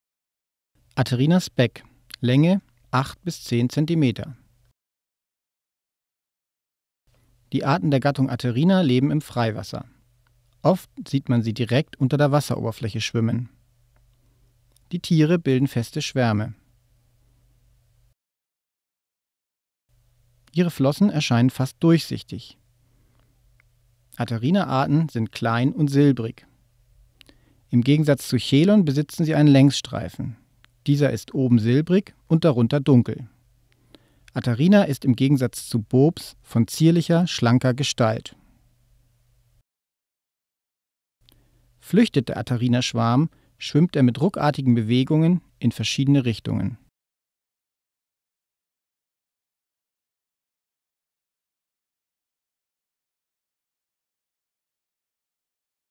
Atherina spec Aehrenfisch
Atherina spec (Ährenfisch) as observed by skin diving at Hotel Cap Roig, Palamos, Spain. Video showing its natural behavior and features for species identification